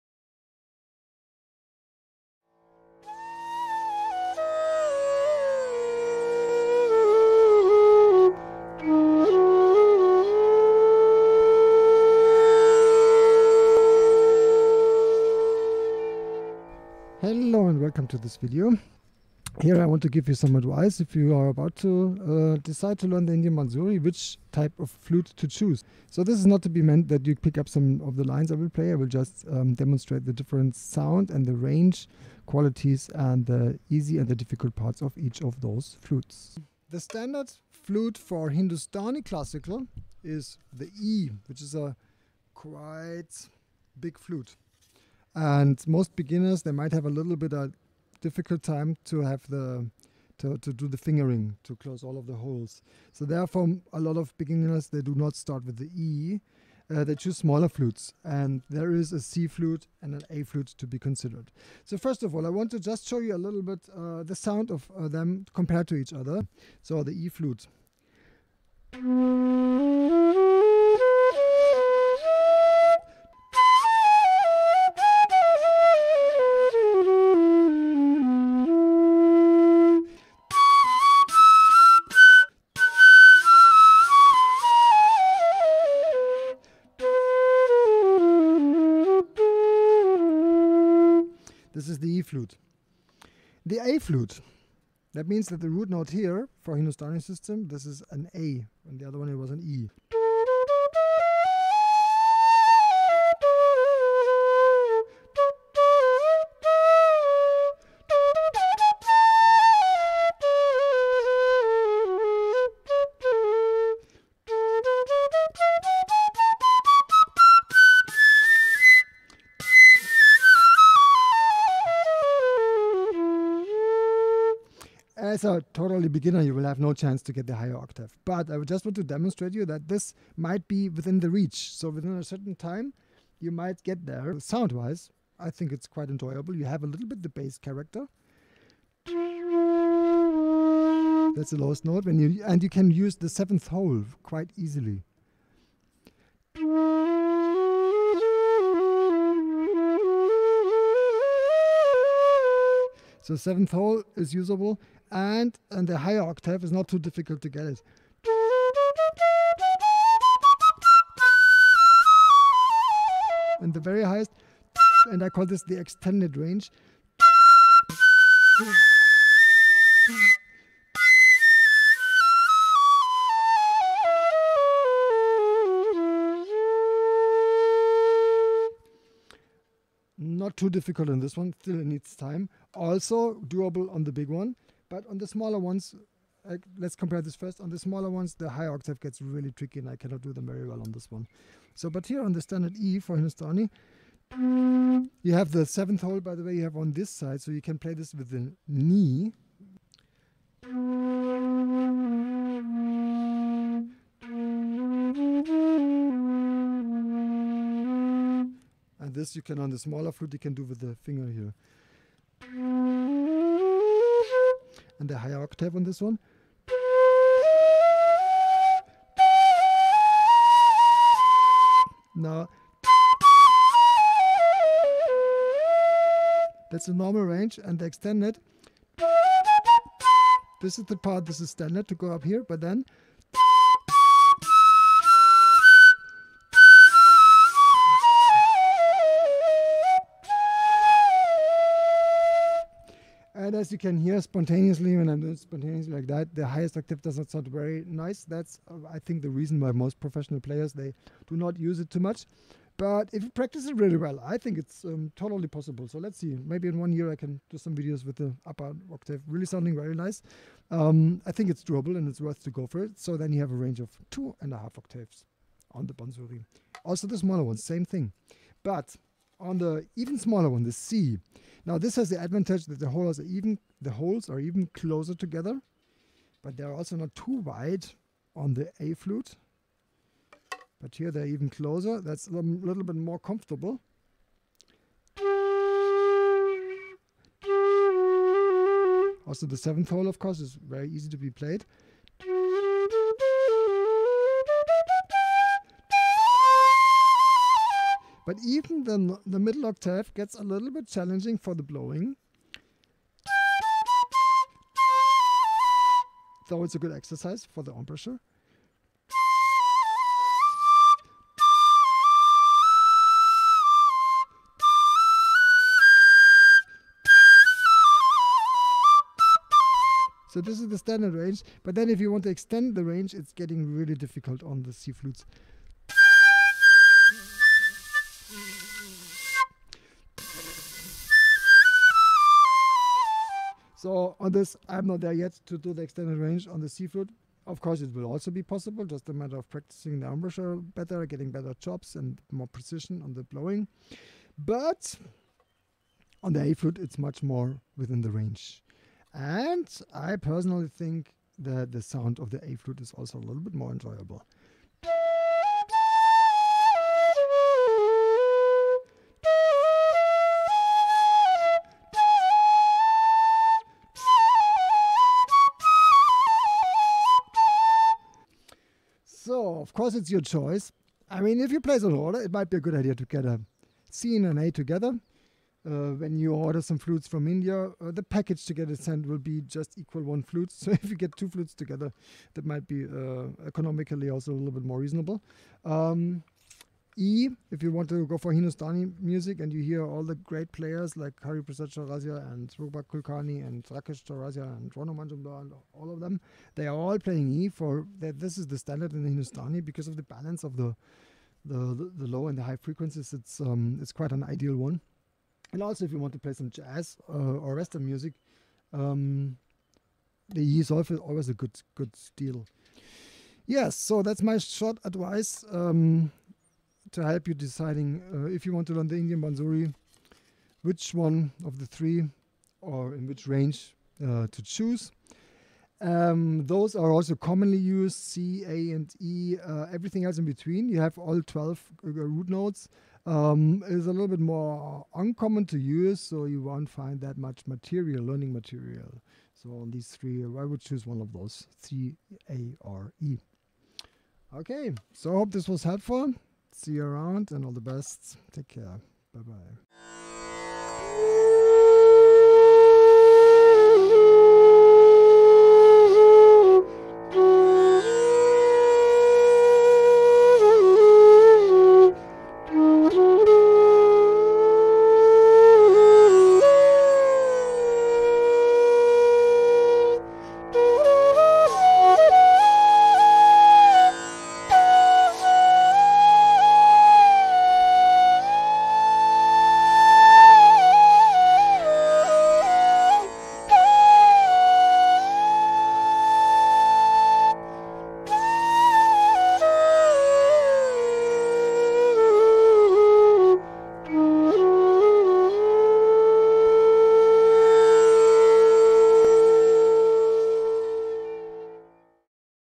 Bansuri Hints for beginners: Which flute to choose
Jazz & Hindustani Music on the Indian Flute

This video is to help beginners on the Indian Bamboo Flute, the Bansuri, to choose the size of flute to start with.